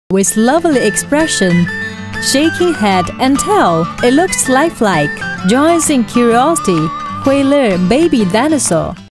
Iguana Interactive Miniature Automated Talking Dino Activity Toy HOLA6105
-When left alone, it snores a couple times before going silent.
-Highly educational learning dinosaur toy for the younger kids
-Very interesting and attractive movements - Talking teaching and learning, pressing buttons etc.
-Bump and go action, the dinosaur can walk around by it self
-Durable quality toy and safety tested for toddler kids